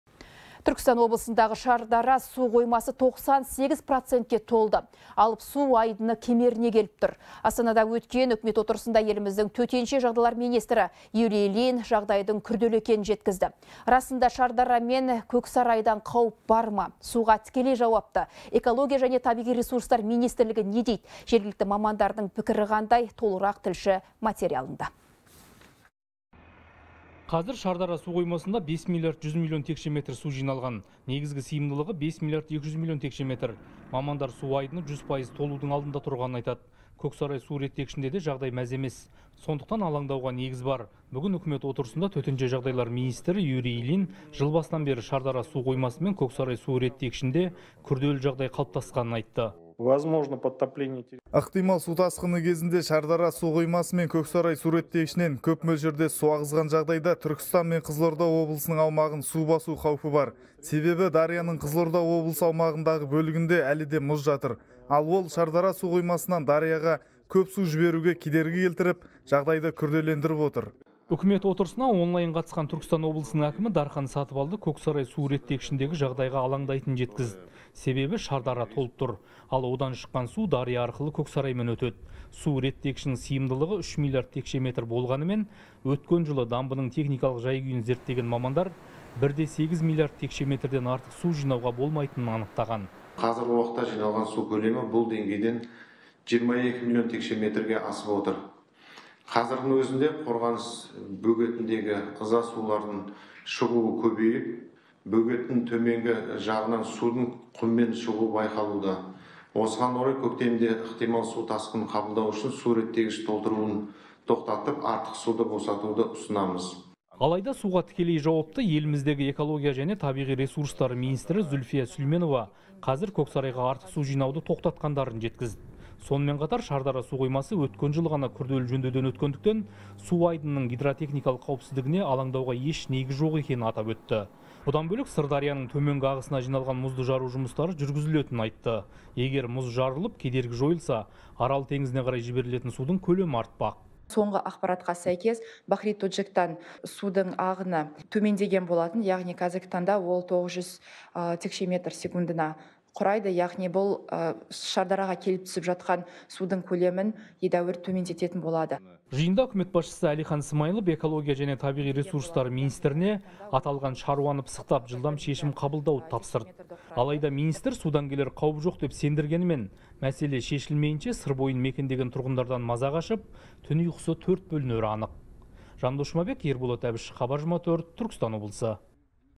Түркістан облысындағы «Шардара» су қоймасы 98% толды
Алып су айдыны кемеріне келіп тұр. Астанада өткен Үкімет отырысында еліміздің Төтенше жағдайлар министрі Юлин Ильин жағдайдың күрделі екенін жеткізді. Расында Шардара мен Көксарайдан қауіп бар ма? Суға тікелей жауапты Экология және табиғи ресурстар министрлігі не дейді? Жергілікті мамандардың пікірі қандай?